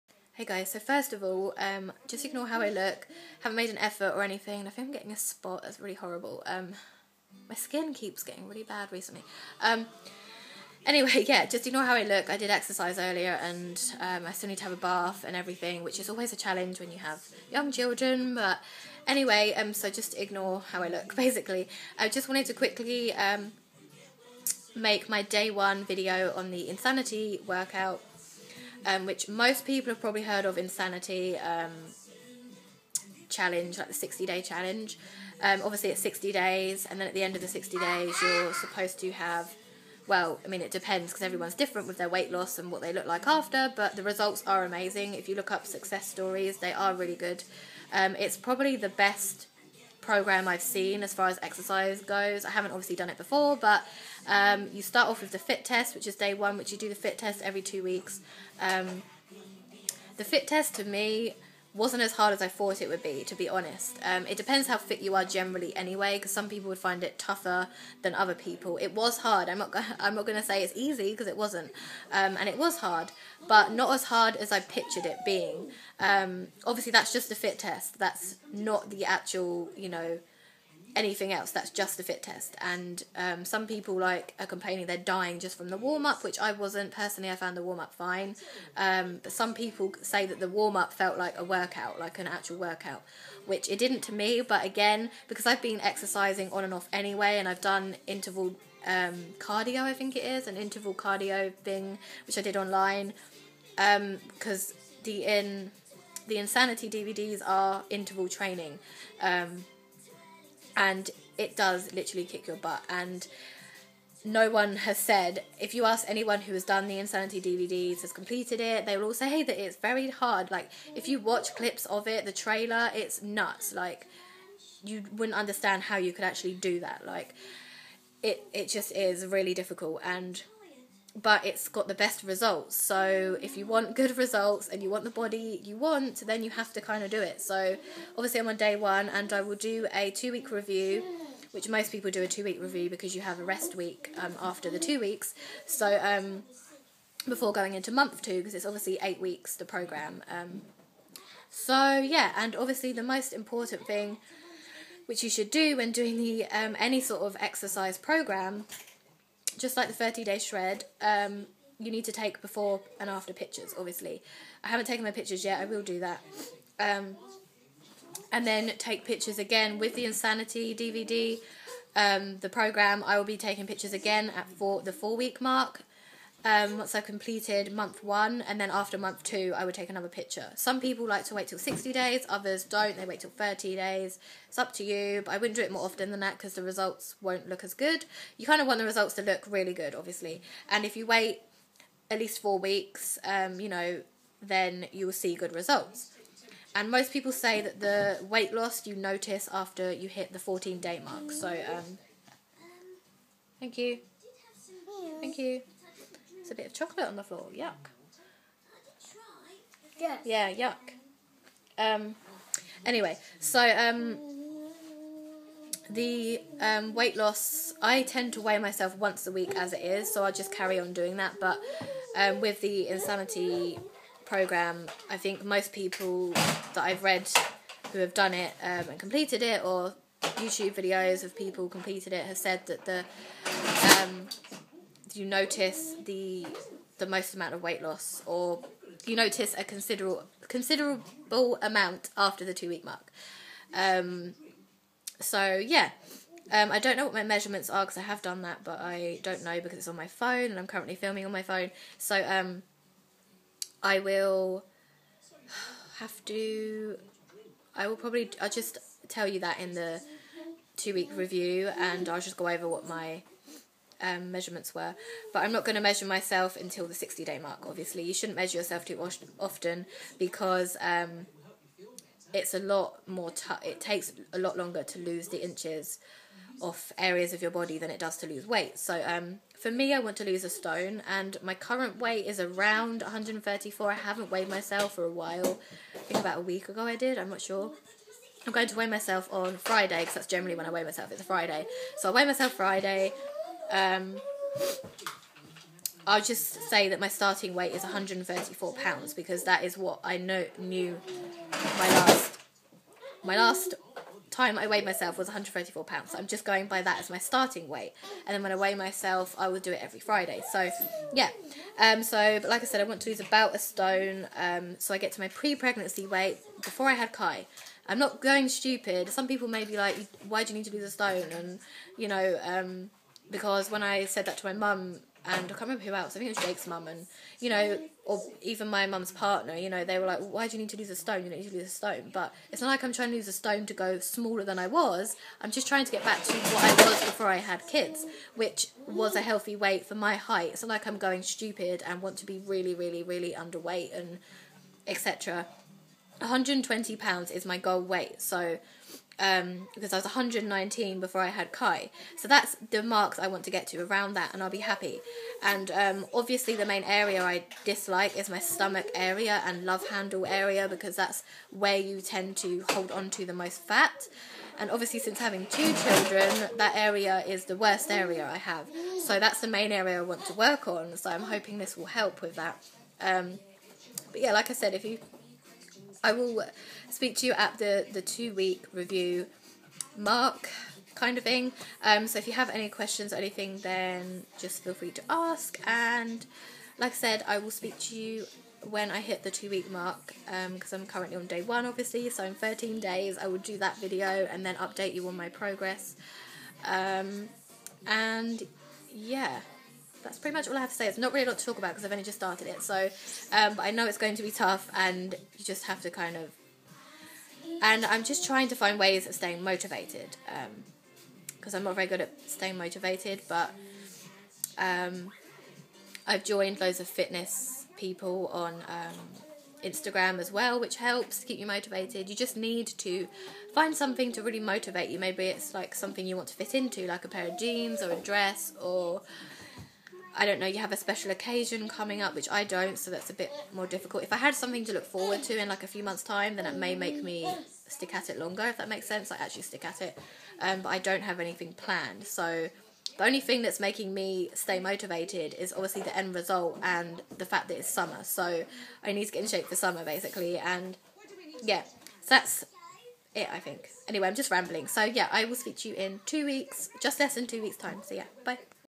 Insanity day 1
Starting the 60 day challenge i will do a 2 week review next and update you on my progress :)